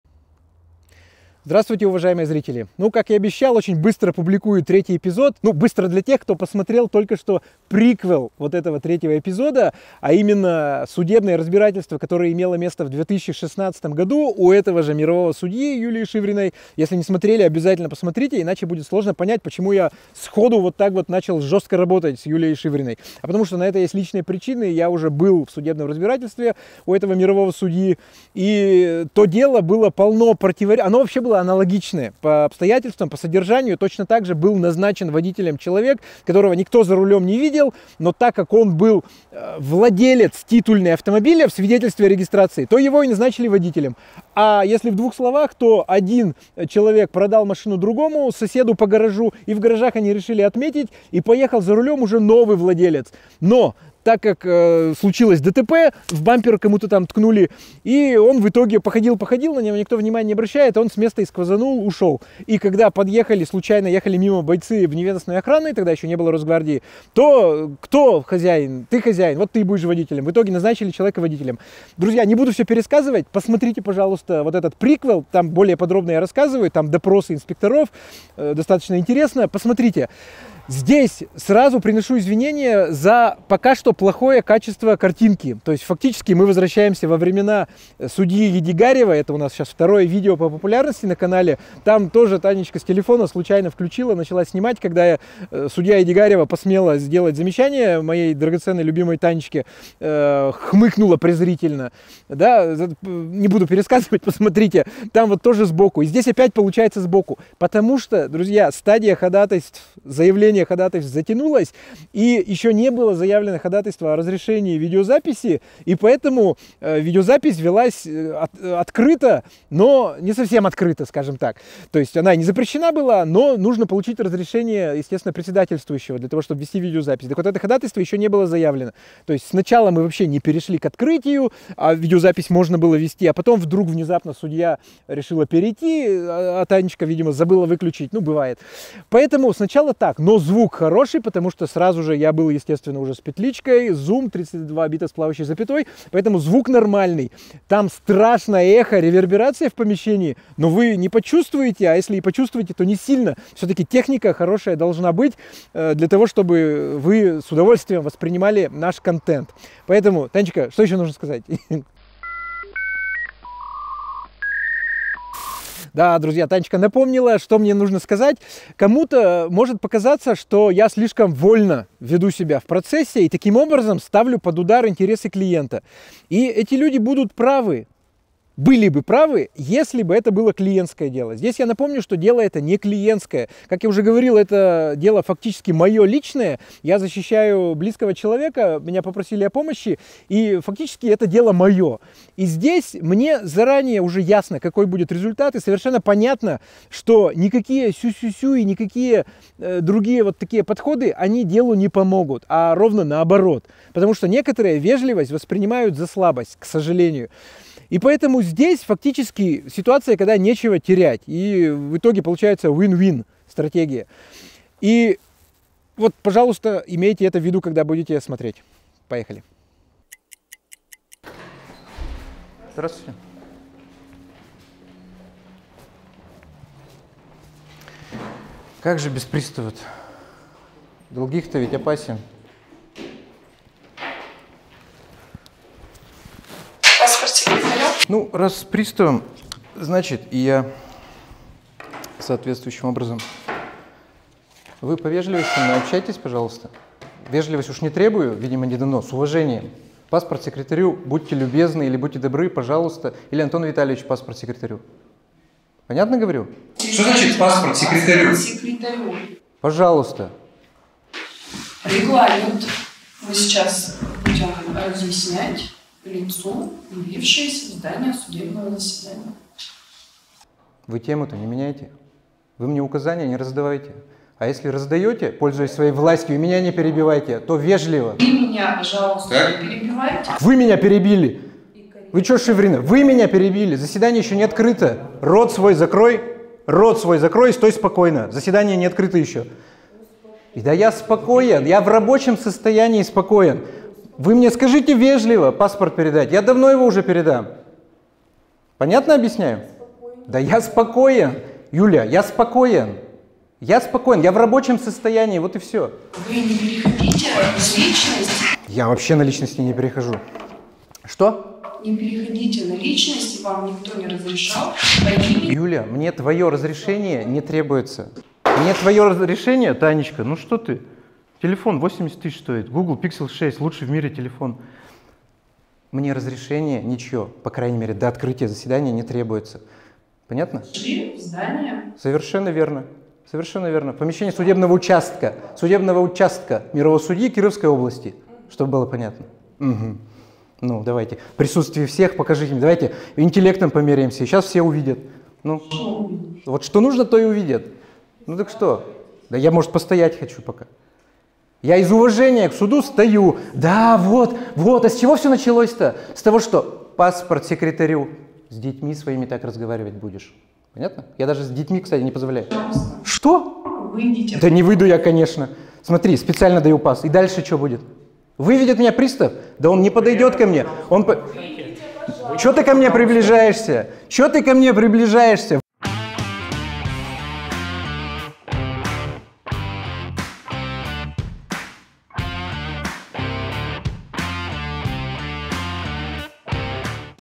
▶️ СПУСТИЛ СУДЬЮ ЮЛЮ С НЕБЕС! 🔥 Досталось и приставу, и прокурору... 👍 НЕ ПОВТОРЯТЬ!
Осуществляю защиту в сложном деле: ДТП, один водитель уехал с места ДТП, второй участник ДТП позвонил друзьям. Обходя район, друзья нашли похожий автомобиль, стоящий под окнами небольшого дома. Постучали в дверь квартиры, под окнами которой стоял автомобиль. Узнав о том, что стоящий под окнами автомобиль принадлежит жене открывшего дверь мужчины, они ворвались в квартиру, несколько раз ударили мужчину, силой вытащили его на улицу.

Прибывшим через час инспекторам ДПС друзья сказали, что вытащенный ими из квартиры мужчина был "вторым водителем" — они опознали его "по описанию", которое дал им их друг (участник ДТП), а именно — по одежде (в которую был одет владелец автомобиля)...

Основываясь лишь на объяснениях друзей второго участника ДТП инспектор ДПС Денис Вохмянин возбудил дело по ч. 2 ст. 12.27 КоАП РФ ("оставление места ДТП"), а инспектор ДПС Денис Исупов возбудил дело по ч. 3 ст. 12.27 КоАП РФ ("пьянка после ДТП"): мужчина был в состоянии опьянения, но т.к. "за рулём" его никто не видел, то Исупов решил "сфабриковать" именно ч. 3 ст. 12.27 — "пьянка после ДТП".

Эта история будет длиться долго. Скорее всего, придётся дойти до Верховного Суда.